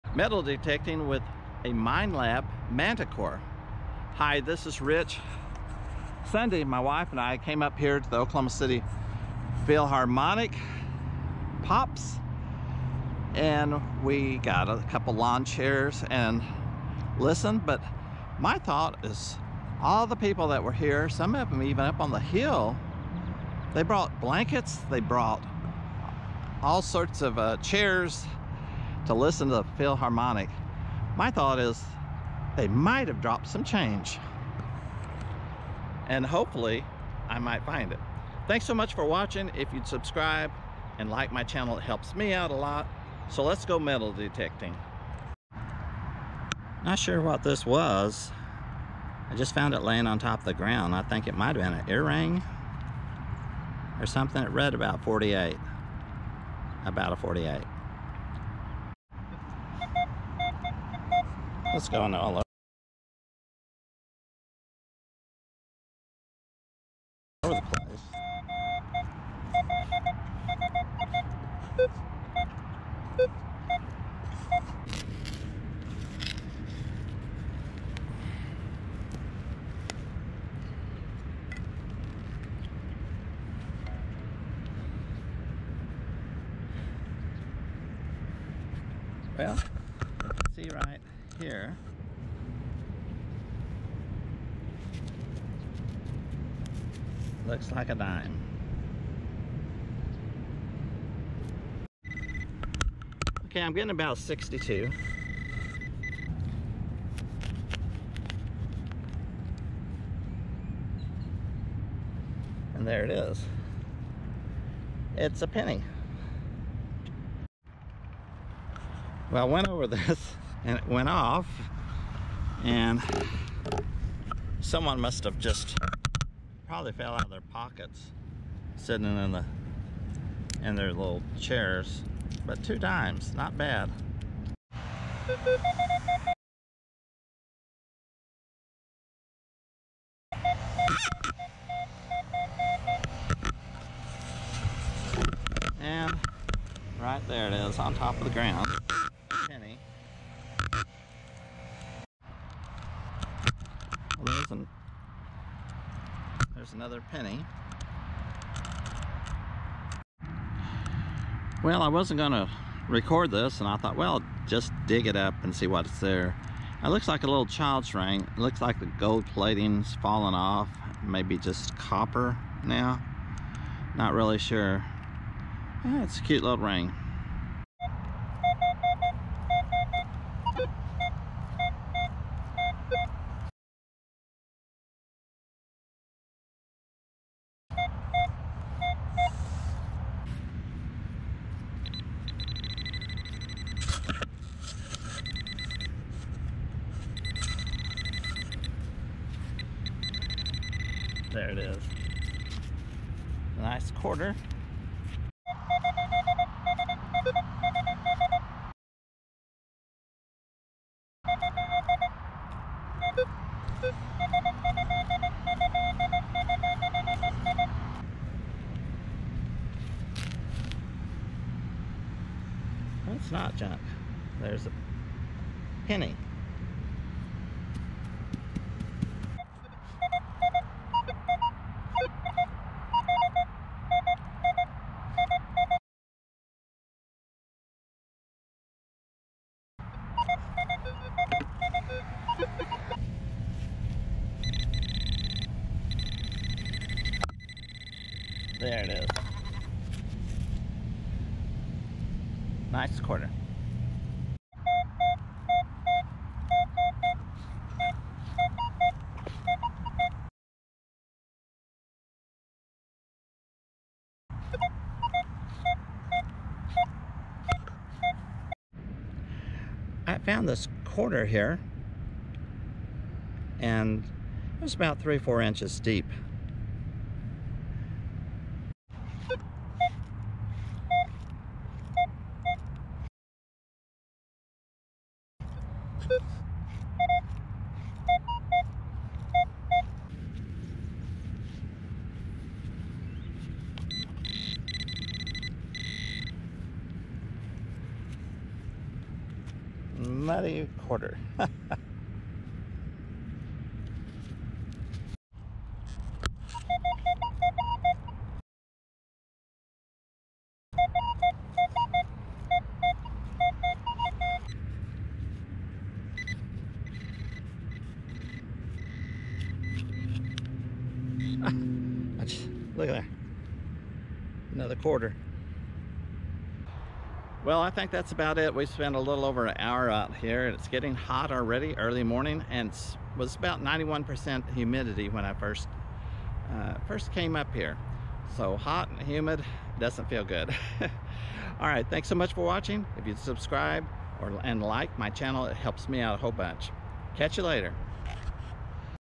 Minelab Manticore OKC Park Metal Detecting
Minelab Manticore OKC Park Metal Detecting.   Metal detecting is fun.  It is more fun when you are using a Minelab Manticore.  I was at a park in OKC seeing what I could find metal detecting.